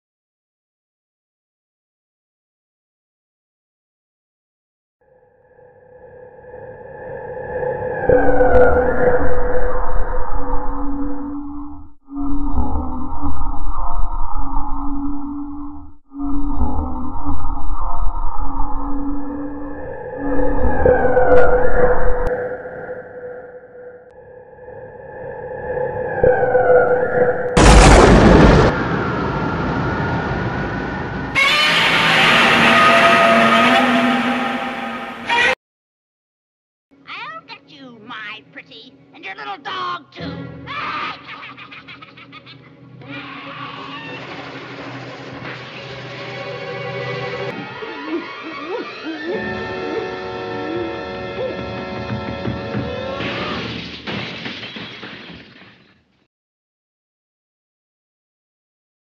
Marc Garcia: Genre Change Project
A project assigned by Audio Video Production to change a movie into a different genre.